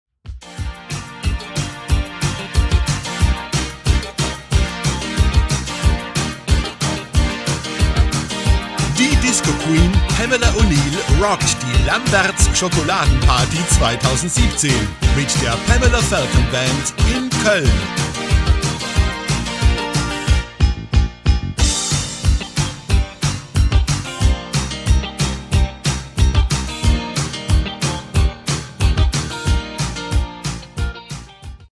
DISCO QUEEN Pamela O'Neal Die Lambertz Monday Night
Erleben Sie die Disco 80er Jahre mit der originalen "Soul Sister" aus Kalifornien, Pamela O'Neal!
Die Disco Queen bieten Ihnen echte amerikanische Soul- und Disco Musik für einen rundum fantastischen Abend. Als "Disco Diva" präsentieren wir Ihnen eine Show, die Sie nicht verpassen sollten.
Mit einer aufwändigen Kostümshow und perfektem "Sound of Disco" zaubern wir Sie in die 70er Jahre zurück oder bieten Ihnen ein Programm mit allen beliebten Klassikern des Soul, Disco und Funk bis in die 80er Jahre..
Erleben Sie die Wiederauferstehung des Disco der 70er, 80er Mit die Disco Diva, Pamela O'Neal